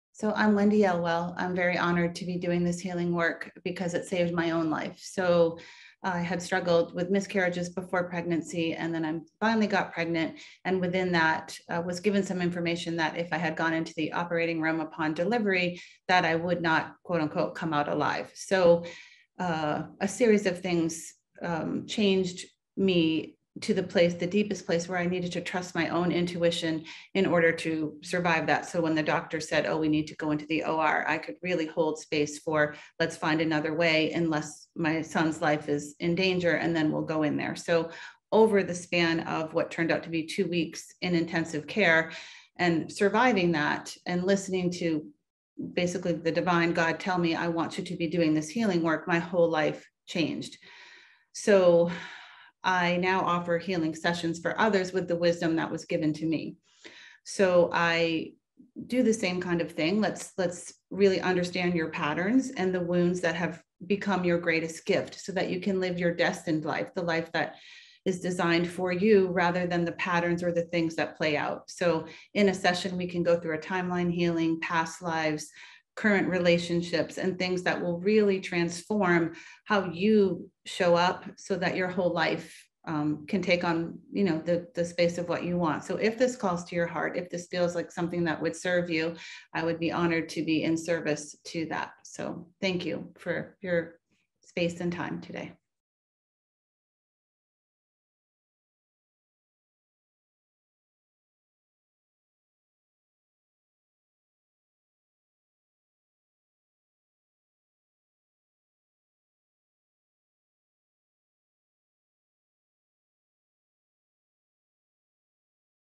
Meet Energy Practitioner Wendy
Meet energy practitioner Wendy,  who talks about why she became an energy practitioner and what she offers in her client sessions